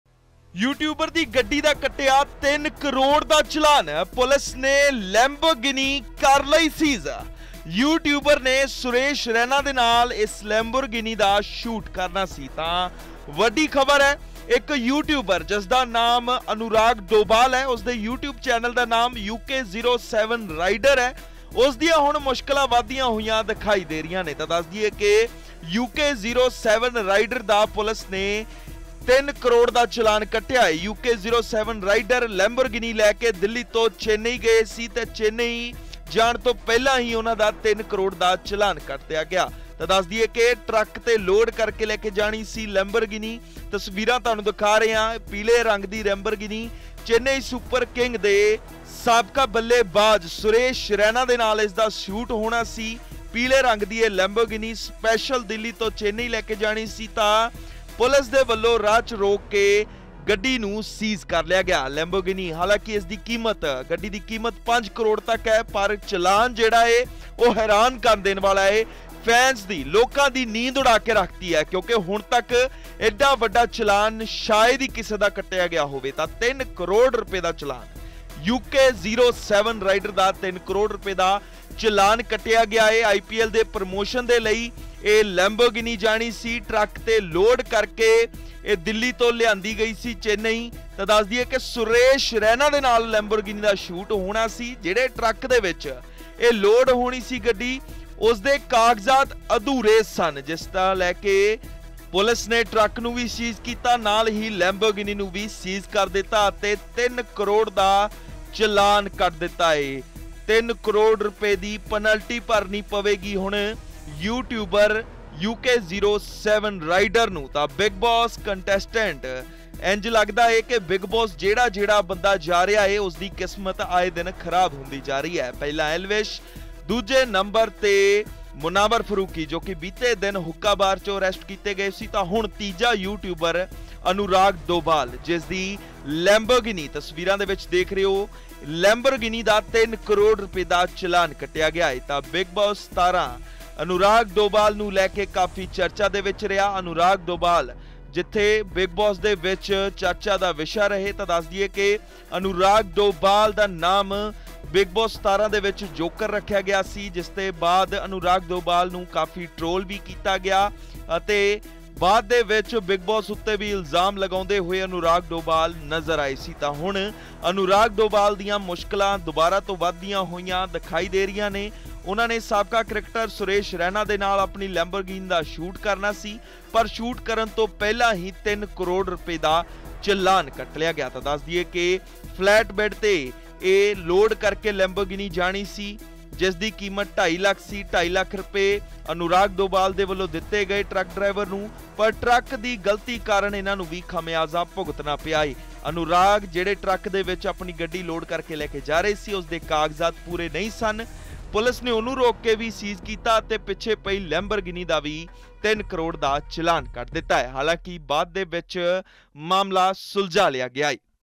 UK07 Rider Anurag Dobhal | ਯੂਟਿਊਬਰ ਦੀ ਗੱਡੀ ਦਾ ਕੱਟਿਆ 3 ਕਰੋੜ ਦਾ Challan ! Lamborghini ਕੀਤੀ ਸੀਜ਼ !
ਯੂਟਿਊਬ ਦੀ ਗੱਡੀ ਦਾ ਕੱਟਿਆ 3 ਕਰੋੜ ਦਾ ਚਲਾਨ ! ਪੁਲਿਸ ਨੇ ਲੈਂਬੋਰਗਿਨੀ ਕੀਤੀ ਸੀਜ਼ ! ਯੂਟਿਊਬ ਨੇ ਸੁਰੇਸ਼ ਰੈਨਾ ਨਾਲ ਕਰਨਾ ਸੀ ਲੈਂਬੋਰਗਿਨੀ ਦਾ ਸ਼ੂਟ !